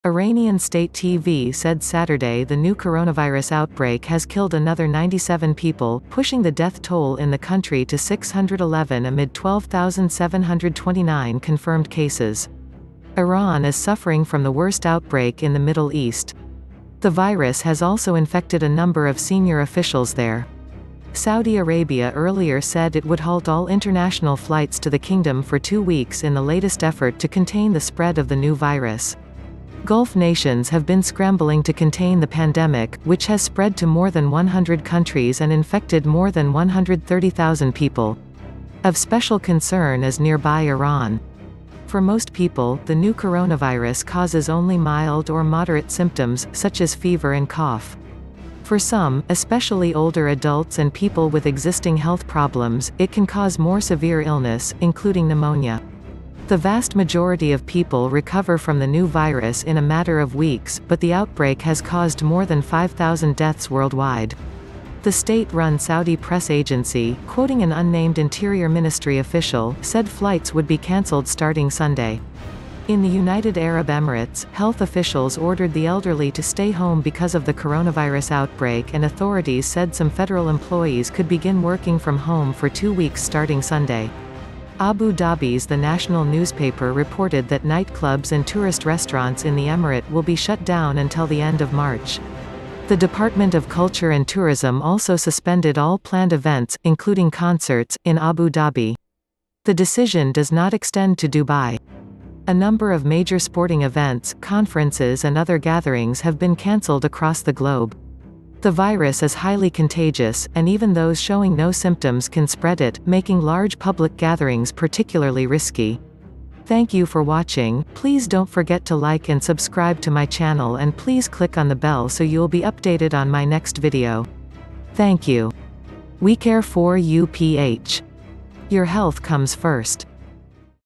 UPDATE: Coronavirus pandemic | Iran says virus kills another 97, pushing death toll to 611
WeCare 4U PH.
Your health comes first!